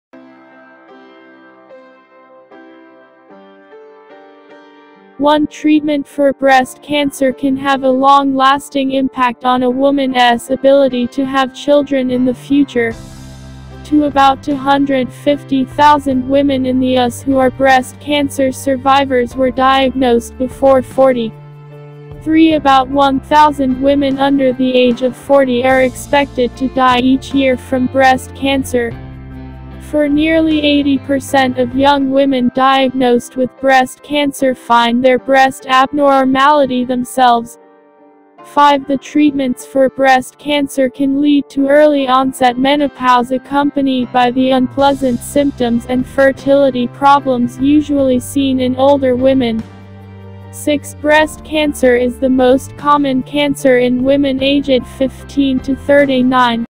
6 Things Young Women May Not Know About Breast Cancer
6 Things Young Women May Not Know About Breast Cancer
1. Treatment for breast cancer can have a long-lasting impact on a woman’s ability to have children in the future.
2. About 250,000 women in the US who are breast cancer survivors were diagnosed before 40.
3. About 1,000 women under the age of 40 are expected to die each year from breast cancer.
4. Nearly 80% of young women diagnosed with breast cancer find their breast abnormality themselves.
5. The treatments for breast cancer can lead to early onset menopause accompanied by the unpleasant symptoms and fertility problems usually seen in older women.
6. Breast cancer is the most common cancer in women aged 15 to 39.